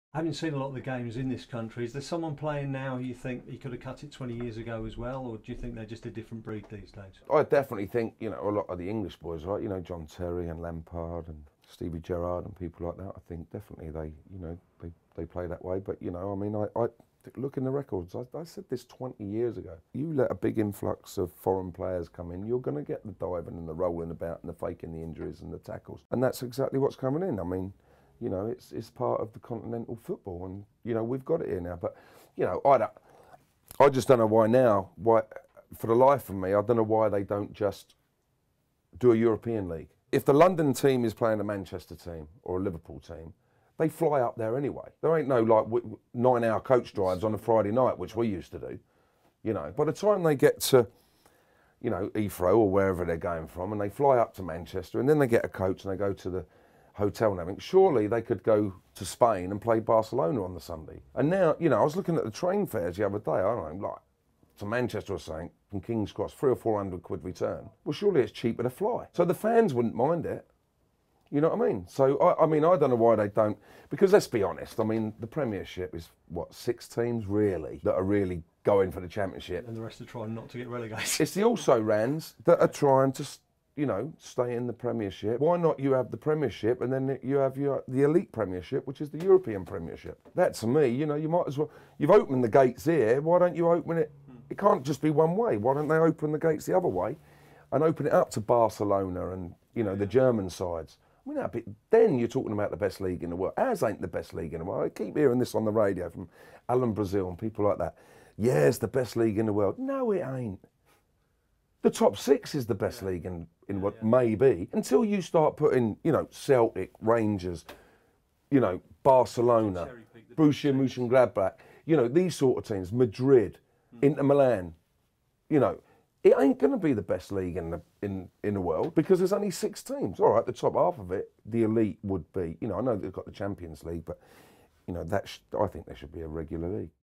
Vinnie Jones on Euro Super League with Celtic and Rangers
VINNIE JONES tells Jim Munro that it's time for the Premier League's top clubs such as Man United, Man City, Chelsea and Arsenal to form a European Super League along with the likes of Barcelona, Real Madrid, Inter Milan. Borussia Moenchengladbach and Scottish pair Celtic and Rangers.